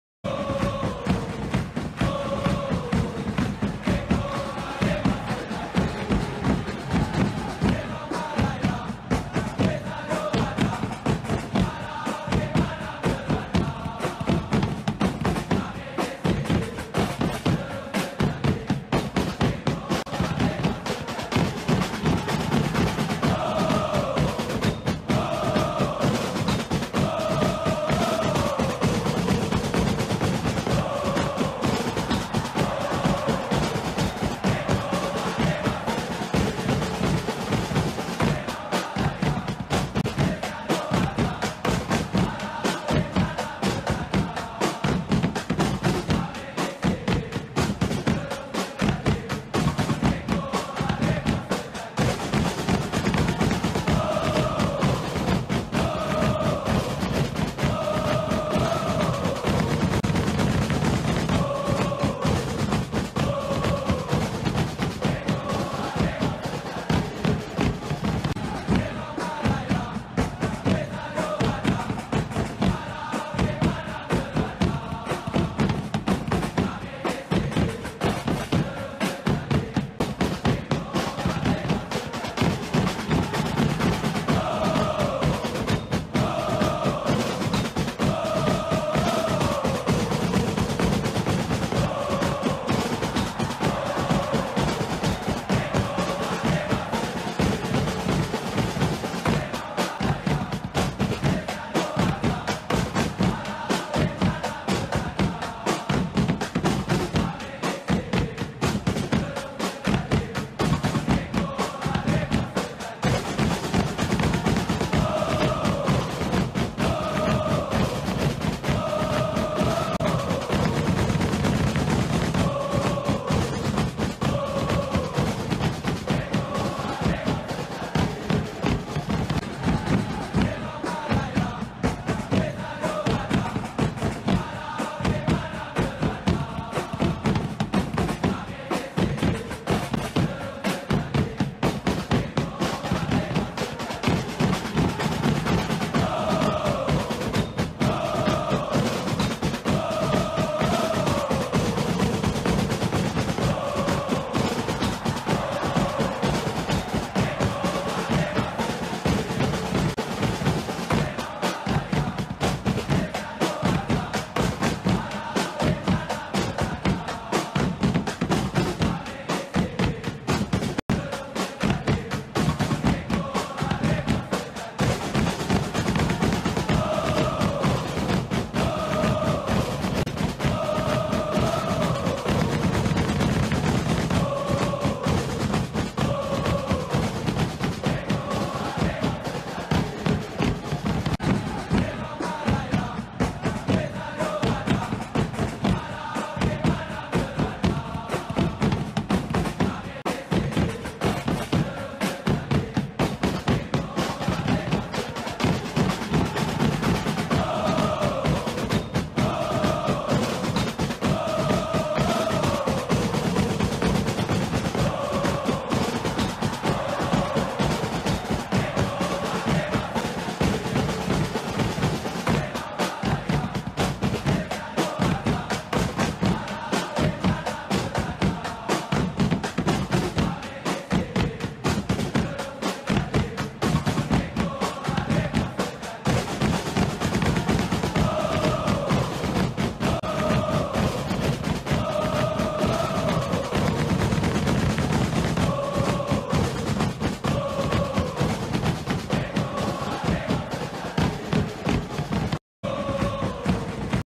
Senarai Pemenang Piala Sumbangsih | Malaysia Charity Shield Winners (1985 - 2022)
Piala Sumbangsih atau juga dikenali sebagai Piala Sultan Haji Ahmad Shah merupakan sebagai penanda pembukaan tirai bagi musim baru liga bola sepak tempatan. Tahukah anda pasukan mana yang paling banyak menjuarai piala ini?